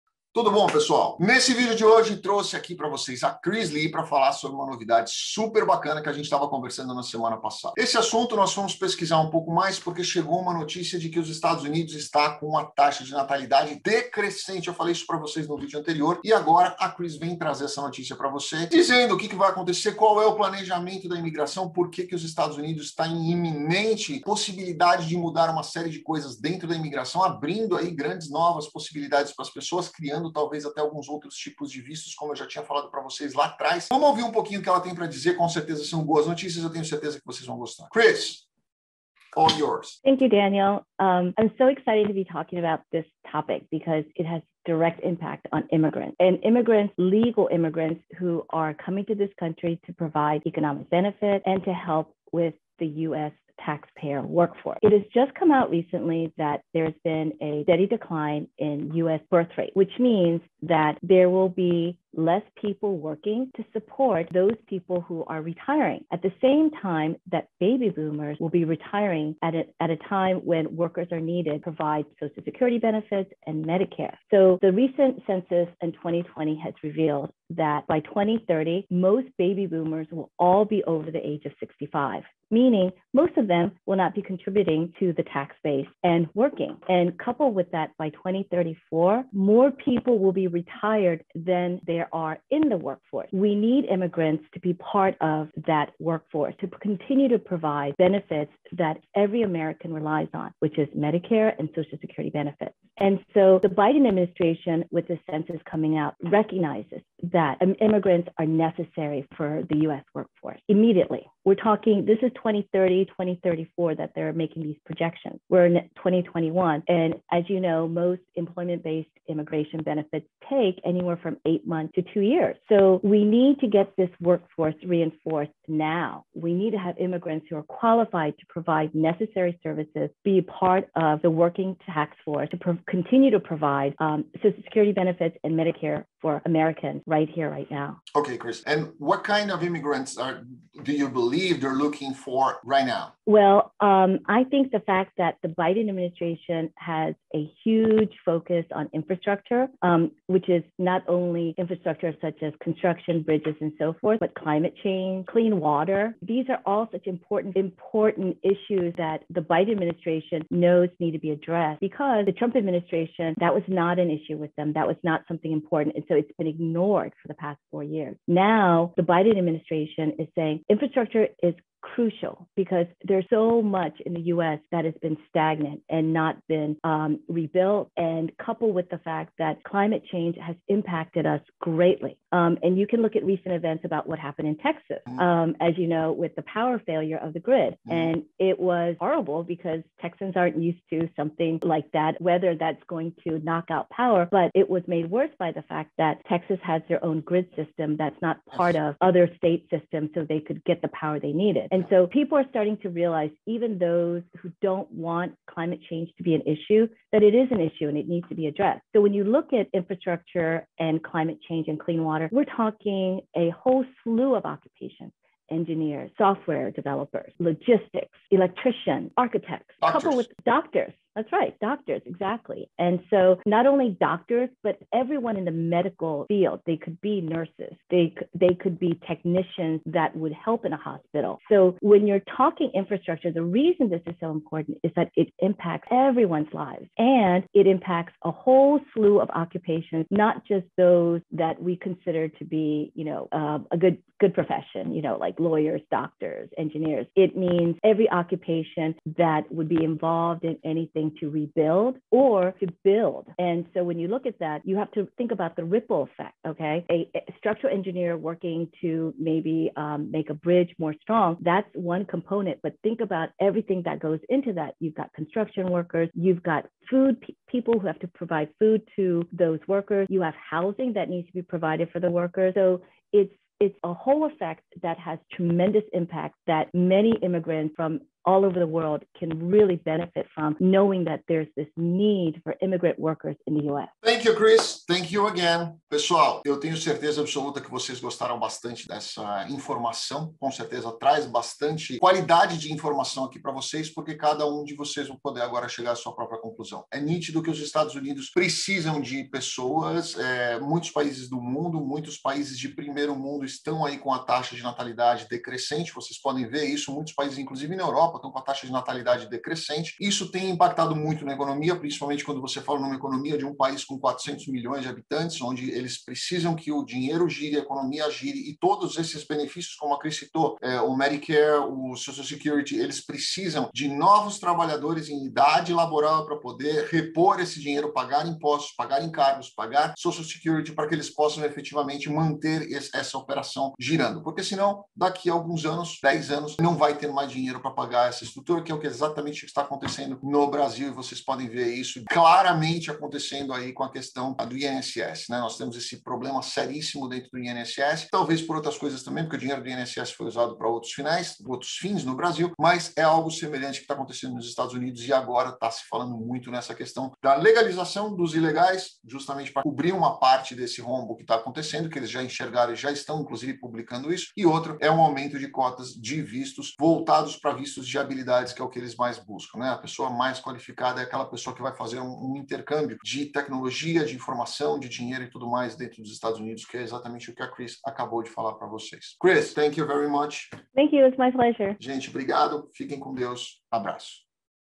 Estados Unidos facilitará visto Americano para Advogados - Engenheiros - Arquitetos e Médicos | EB2
Nesse vídeo Advogado brasileiro Daniel Toledo e Advogada americana Kris Lee sócios da LeeToledo PLLC,  falam sobre a Imigração nos Estados Unidos que pode se tornar mais flexível para profissionais qualificados.

Você conhece o visto americano EB2 ?
E se você realmente precisa dele, e as qualificações necessárias para obtê-lo.
O que é visto americano eb2? Saiba se você se qualifica para tirar o visto americano EB2
O visto EB-2 é um visto americano destinado a profissionais estrangeiros experientes que concede residência permanente ao requerente.

Como funciona o visto eb2?
O EB-2, em qualquer de suas modalidades, inclusive o NIW – aquele concedido em razão do Interesse Nacional Americano, quando aplicado e aprovado, dá o direito ao Green Card, ou seja, residência permanente nos Estados Unidos para o beneficiário, seu cônjuge e filhos menores de 21 anos.

Existem vários tipos de visto americanos:

⭐ VISTO EB-3: Visto para trabalhadores profissionais qualificados.
⭐ VISTO EB-5: Visto para investidores
⭐ VISTO L-1: Visto para empresários atuarem nos Estados Unidos
⭐ VISTO P: Vistos para atletas e vistos para artistas